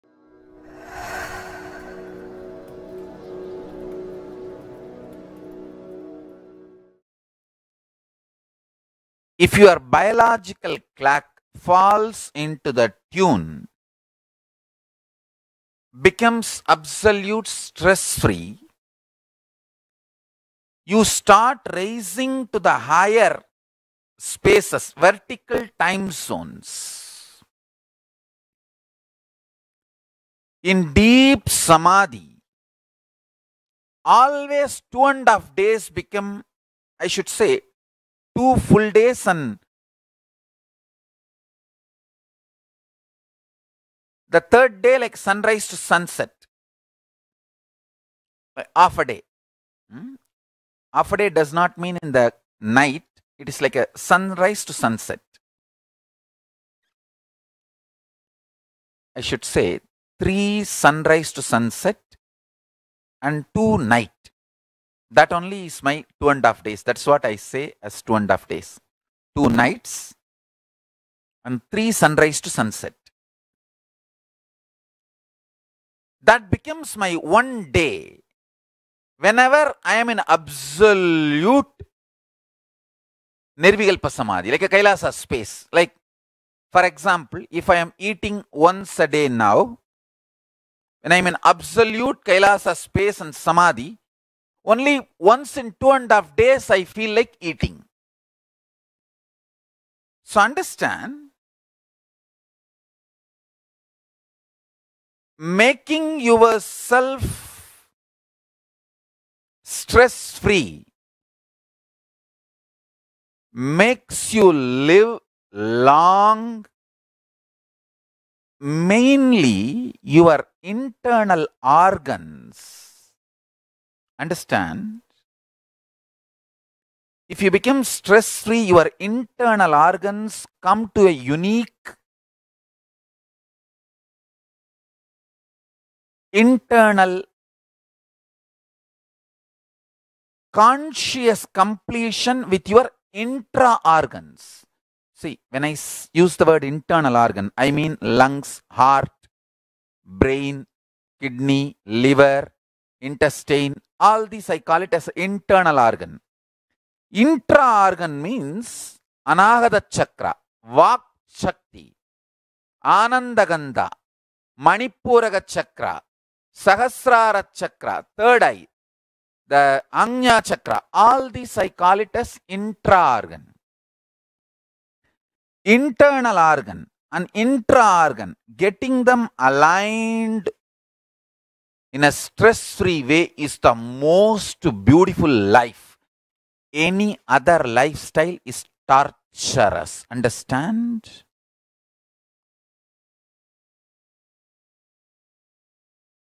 Tune Your Biological Clock Towards the Higher Time Zones
If your biological clock falls into the tune, becomes absolute stress free you start raising to the higher spaces vertical time zones. In deep samadhi always two and half days become, I should say two full days and the third day like to sunrise to sunset, the half-a-day, half-a-day does not mean in the night, it is like sunrise to sunset, I should say 3 sunrise to sunset and 2 night, that only is my 2 and half day, that’s what I say 2 and half days, 2 nights and 3 sunrise to sunset, that becomes my one day whenever I am in absolute Nirvikalpa Samadhi, like a Kailasa space like, for example, if I am eating once a day now and I am in absolute Kailasa’s space and samadhi, only once in two and half days I feel like eating. So understand making yourself stress free makes you live long, mainly your internal organs, understand, if you become stress free your internal organs come to a unique internal conscious completion with your intra-organs. See when I use the word internal organ, I mean lungs, heart, brain, kidney, liver, intestine all this I call it internal organ, intra-organ means anahata chakra, vaak shakti, ananda gandha, manipuraka chakra, sahasrara chakra, third eye, the ajna  chakra, all this I call it as intra-organ. Internal organ and intra-organ getting them aligned in a stress free way is the most beautiful life, any other lifestyle is torturous, understand.